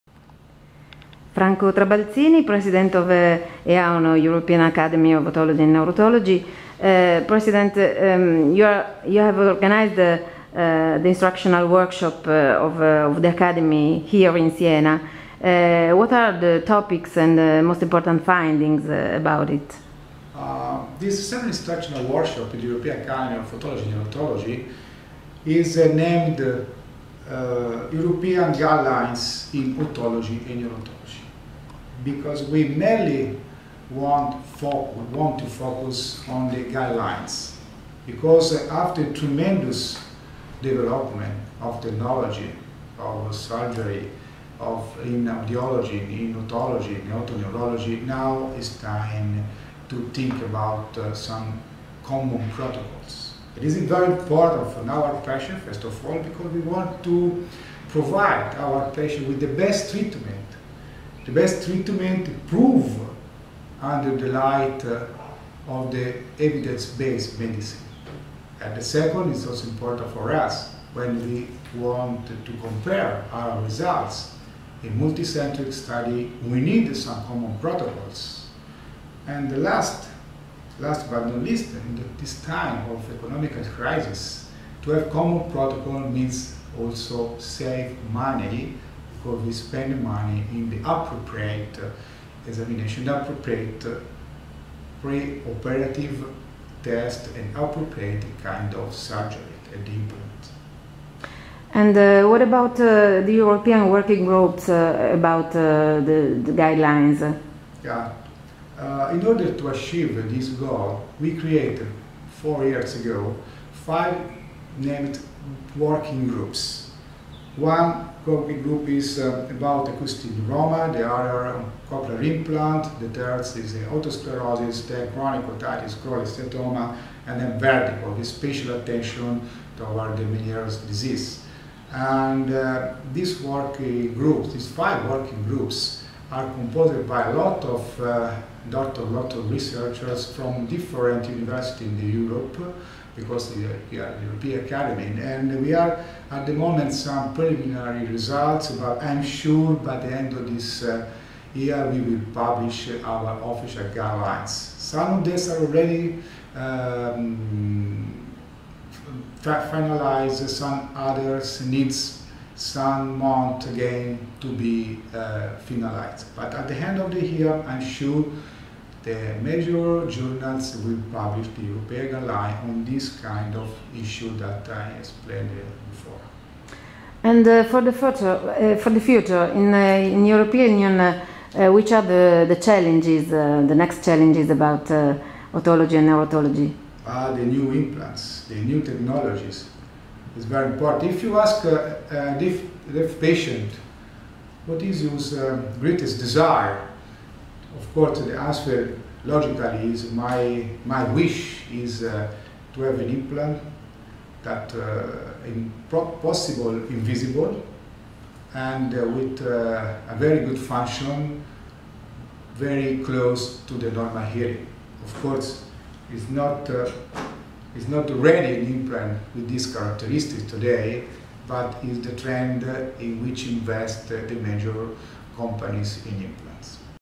Franco Trabalzini on the 7th Workshop EAONO 2014 in Siena (Italy)
Interview with the president 2014 of the European Academy of Otology and Neuro-Otology.